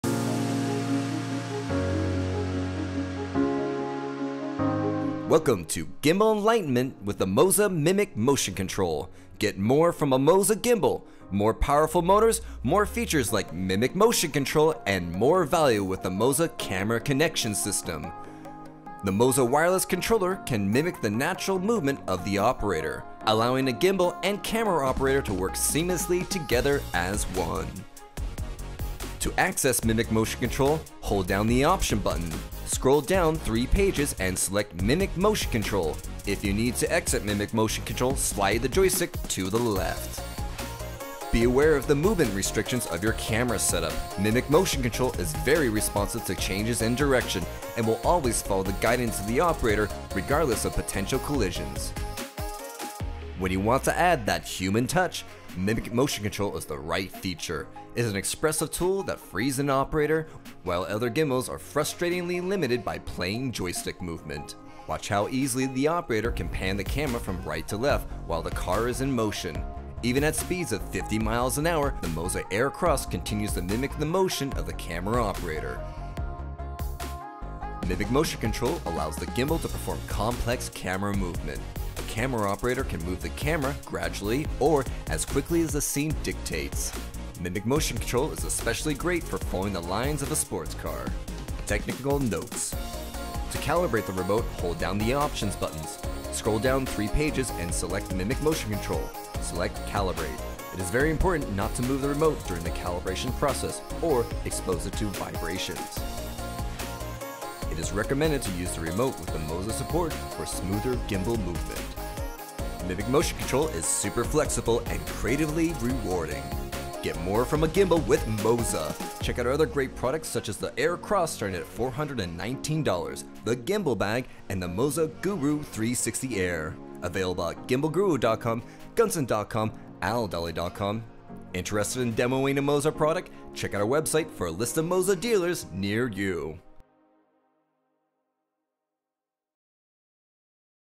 Introduction to Mimic Motion Control with MOZA AirCross
Learn how to setup, calibrate, operate, and more about Mimic Motion Control. Available on MOZA AirCross and MOZA Air with the MOZA Wireless Remote.

Battery Adapter for Sony Alpha Camera, check website for pricing.